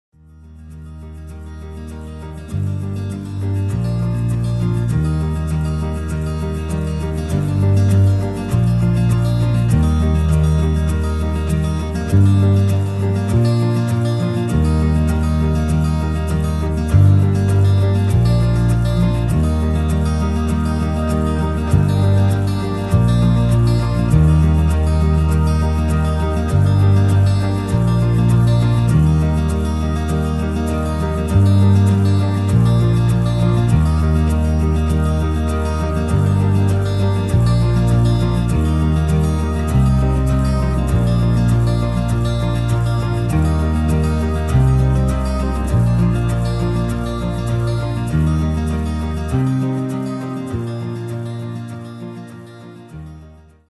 560 m&#178; Land for sale in Western Cape | West Coast | St Helena Bay | Britannia Bay | T993236
560 m² Land available in Britannia Bay, 12 mosselplaat crescent, EXCLUSIVE SOLE MANDATE - BRITANNIA BAAI BEACH ESTATE  This vacant stand of 560 sqm is on level groun